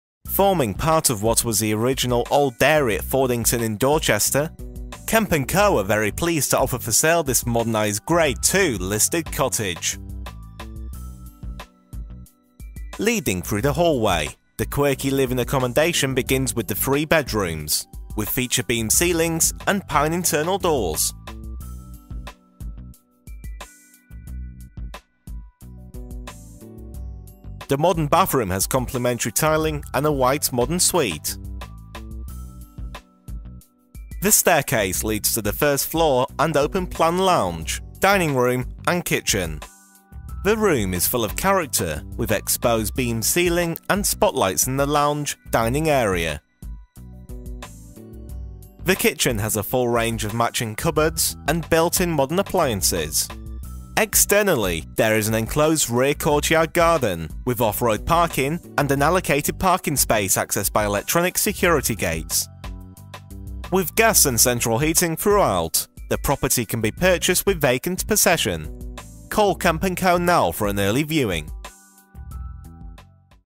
FANTASTIC OPPORTUNITY! To Own This Grade II Listed Cottage in Fordington, Dorset
Forming part of what was the original Old Dairy at Fordington in Dorchester .Kemp and Co are very pleased to offer for sale this modernised Grade II listed cottage

Leading through from the the hallway; ...the quirky living accommodation begins with the 3 bedrooms ...with feature beamed ceilings and pine internal doors

The modern bathroom has complimentary tiling and a white modern suite

The staircase leads to the first floor and the open plan lounge / dining room and kitchen

The room is full of character with an exposed beamed ceiling and spotlights in the lounge / dining area

The kitchen has a full range of matching cupboards and built in modern appliances

Externally; there is an enclosed rear courtyard garden with off road parking and an allocated parking space accessed by electronic security gates.

The Grade II listed cottage is ideally located in the much sought after conservation area of Fordington with great local amenities and rail links to both Waterloo and Bristol Temple Mead.

With gas central heating throughout; this property can be purchased with vacant possession. call Kemp and Co on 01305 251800 now for an early viewing.

TOP TIP!! ALways select the HD option for THE best viewing experience!